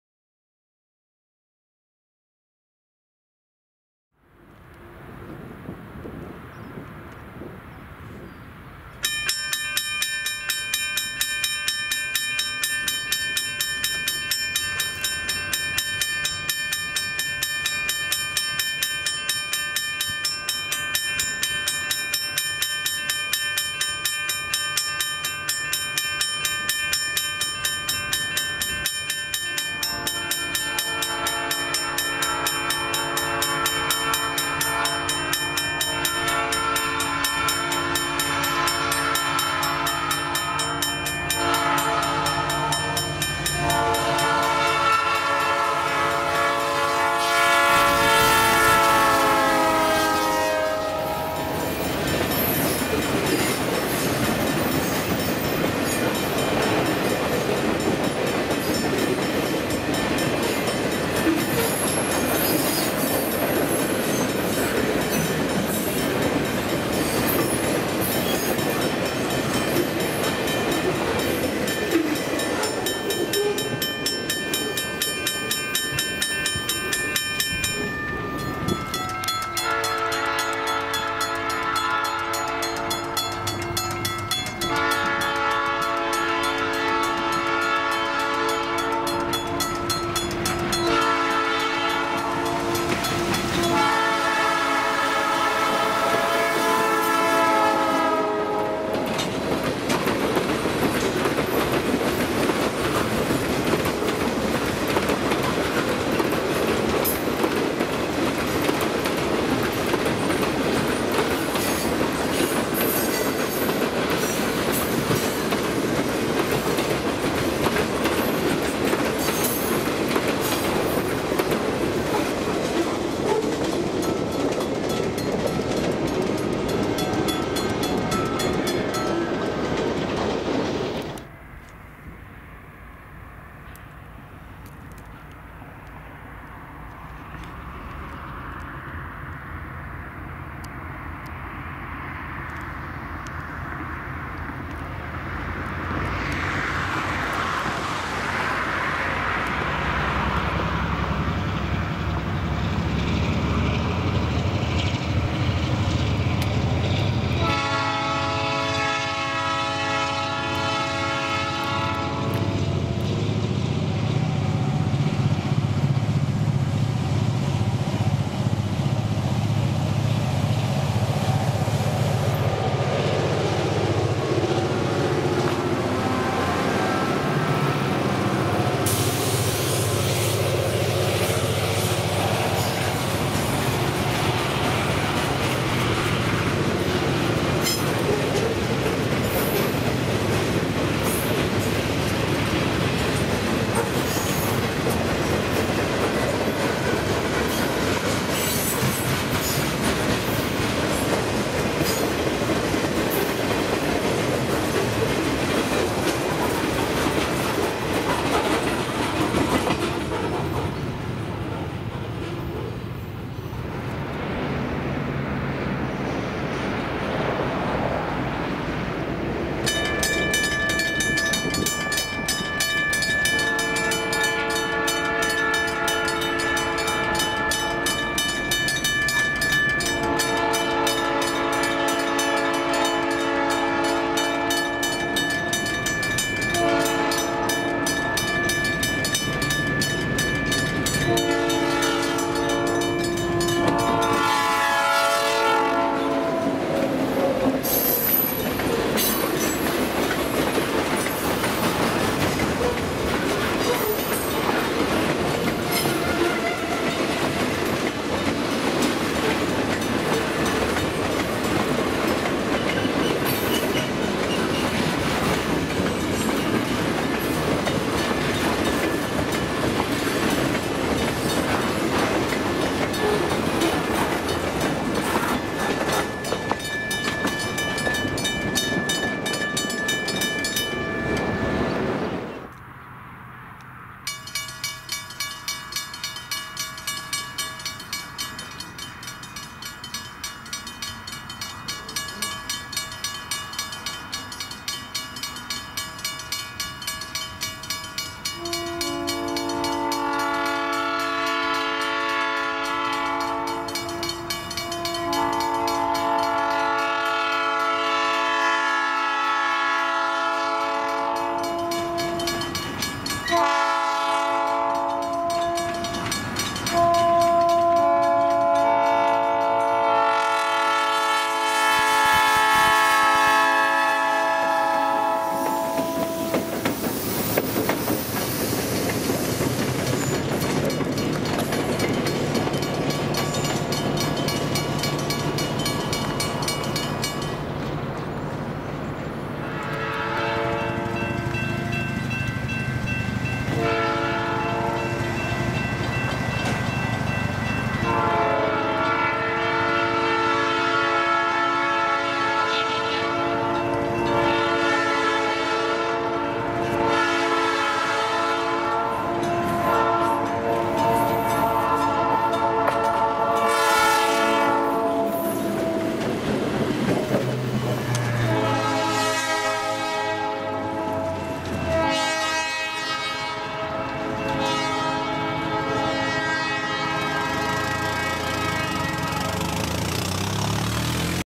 CFNR's Cortena extra on the West Valley line 1-15-17
Location: Yolo County Line Rd, Hershey, CA; Main St, Dunnigan, CA; Co Rd 99W; Dunnigan, CA; Road 93B, Zamora, CA; Co Rd 18, Woodland, CA; Court St, Woodland, CA
Train: Cortena extra
Engines:
CFNR GP15-1 #1568
CFNR GP15-1 #1570